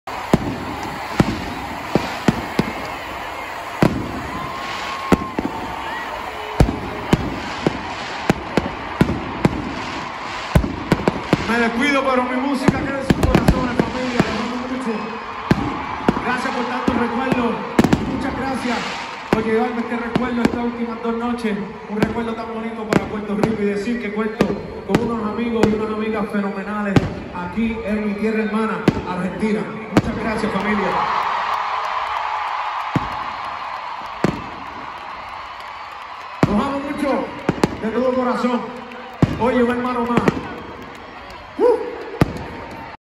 DADDY YANKEE se despidió del público argentino ❤️‍🔥
Palabras fínales de la última noche del artista en Buenos Aires con dos estadios Vélez estallados 🔥